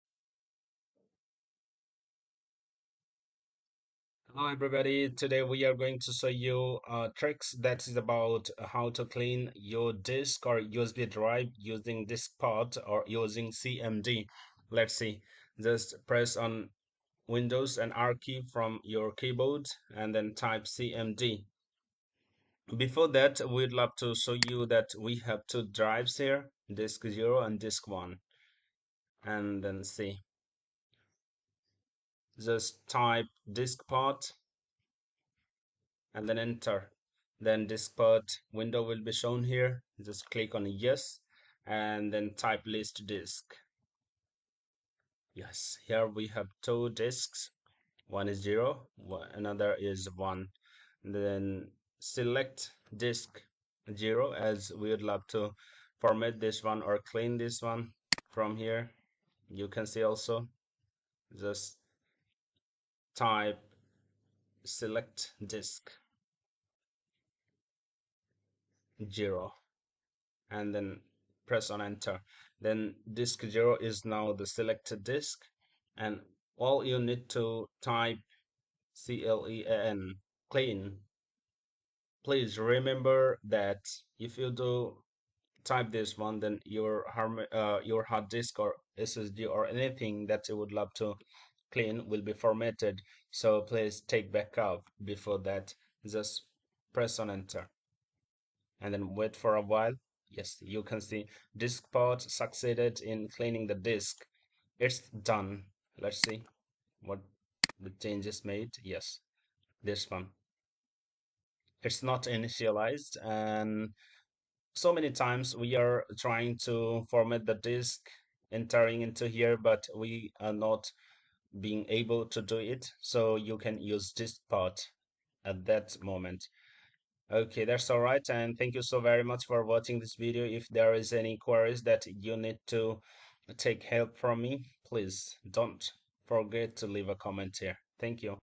Diskpart Tricks| How to format HDD, Pen drive, SSD using CMD?
We are trying to format the disk in many ways, but it is not working every time. Using Diskpart or cmd can help here.
Type cmd in the Run
Type diskpart
Type list disk to see the disks
Type select disk (Disk Number) which you want to clean or format
Type clean

Please remember one thing, keep a backup of your files stored on that disk. After applying these commands, your HDD or the disk will be cleaned, and no data will remain there.